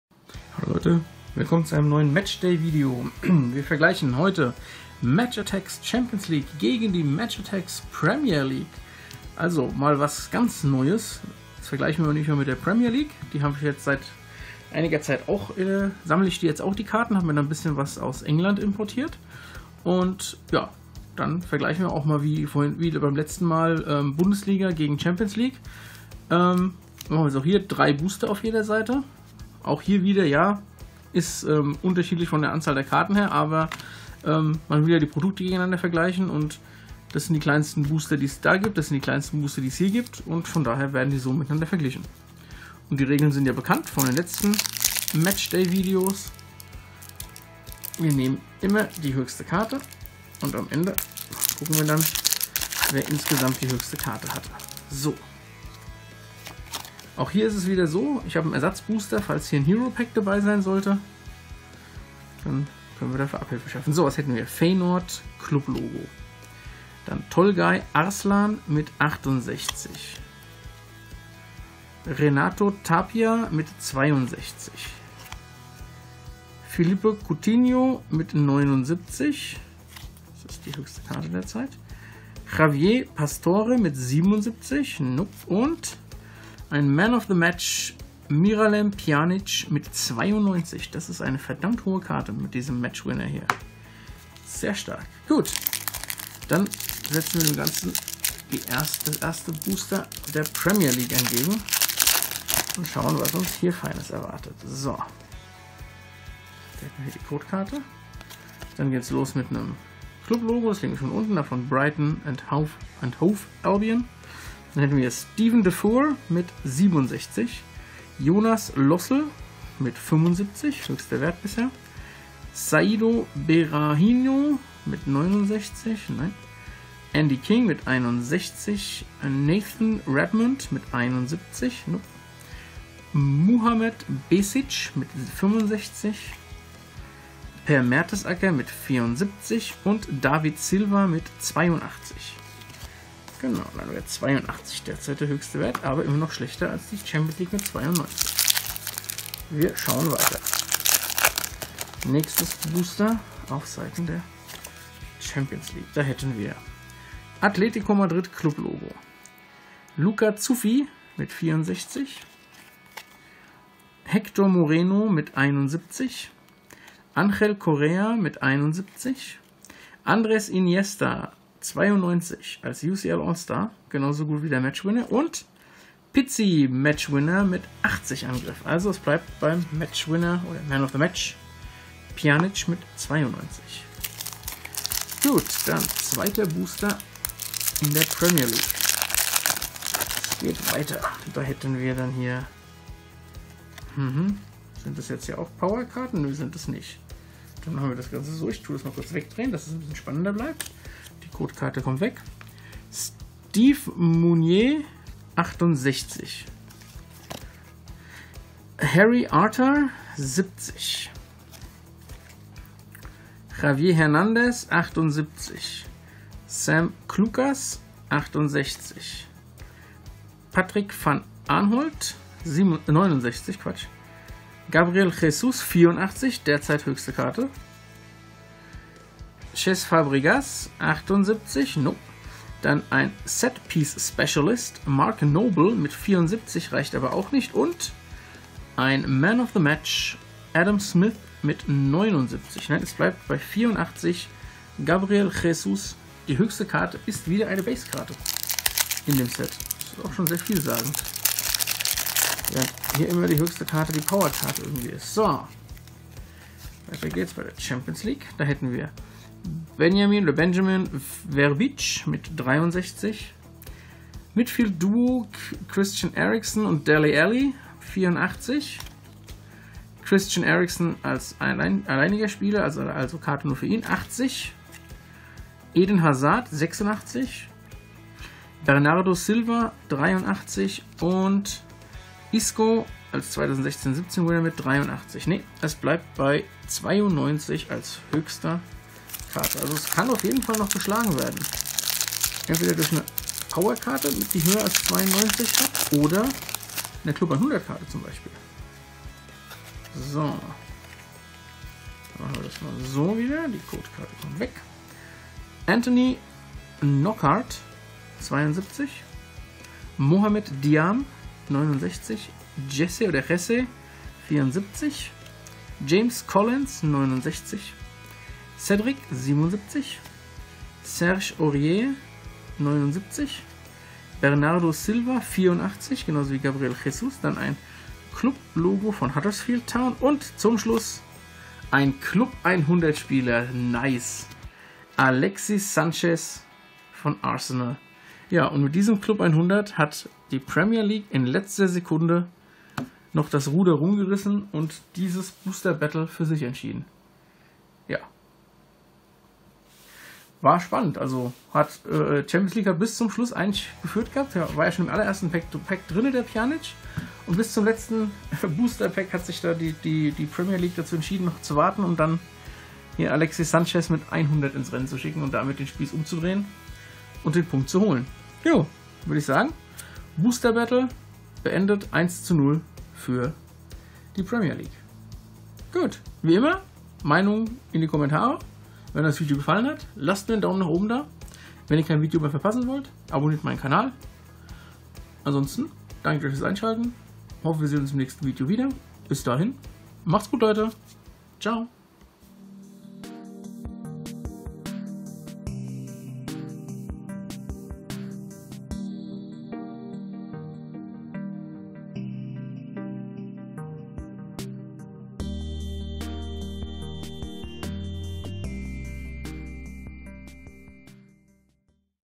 Match Attax 2017/2018 - Champions League vs. Premier League: Booster-Battle
Der Serien-Vergleich geht in die nächste Runde: Heute vergleichen wir die Match Attax Sammelkarten der aktuellen Champions Leage Serie gegen die aktuelle Premier League Serie. Los geht es wieder mit einem Booster-Battle - viel Spaß ;o)

Ihr wollt mir einen Brief und/oder Karten für ein Mailday schicken? Dann benutzt am besten mein Postfach:

Langly1013
Postfach 12 48
65781 Hattersheim